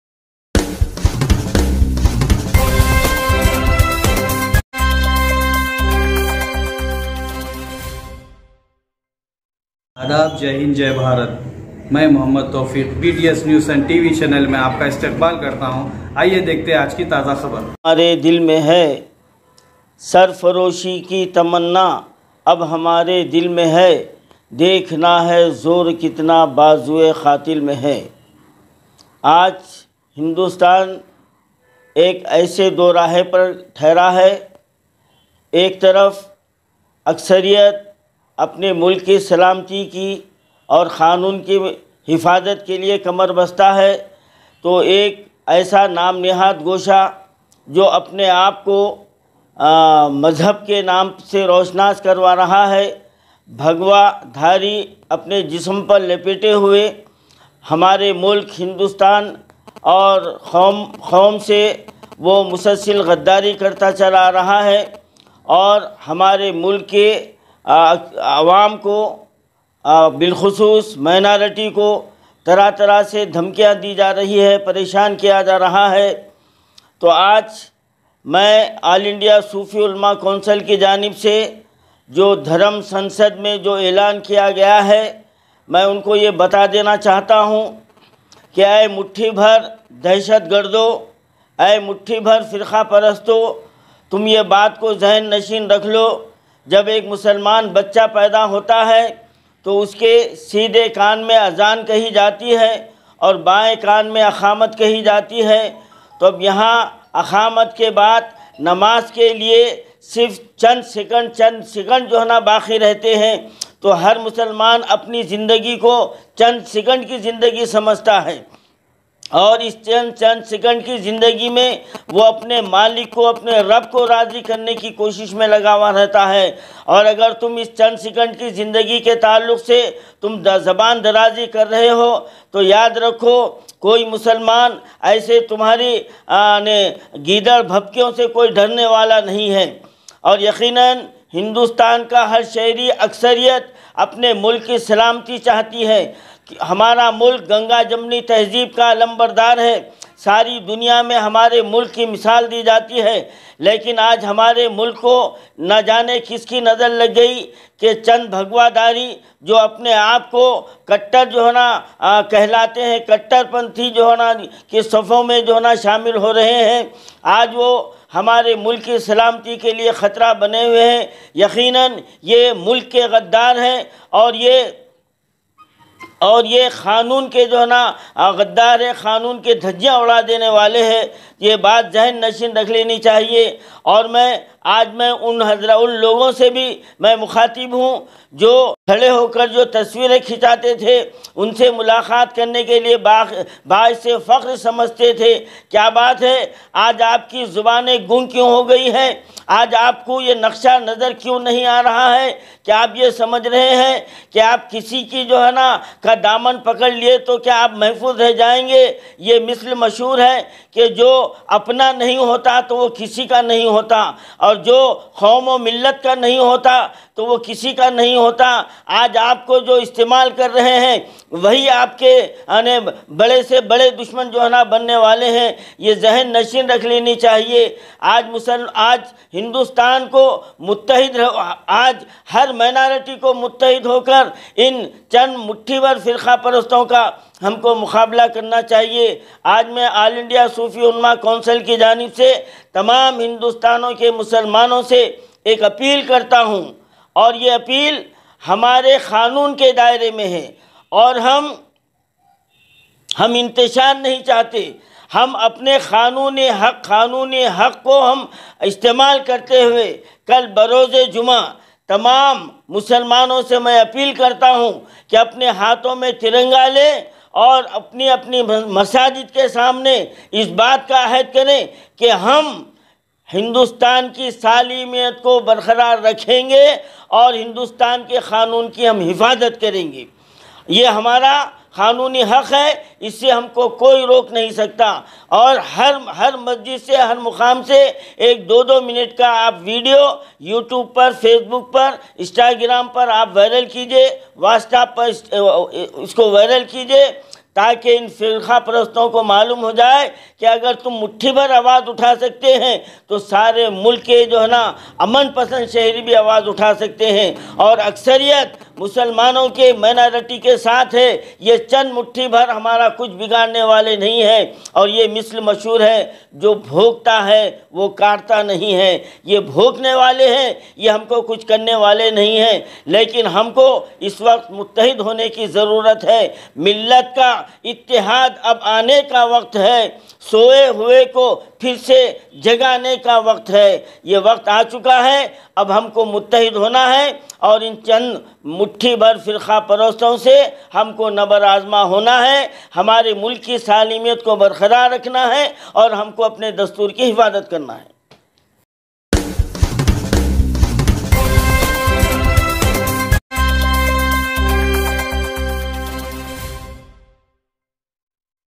Dharam sansat may sharangas dhamki o par adar all India sufi ulooma council janab
Dharam sansat may sharangas dhamki o par

Sadar all India sufi ulooma council janab khairuddin saab qubla ka shaddid itajaji markazi hukmoot qhaqi o k khilaaf saqt karvai ka mutalaba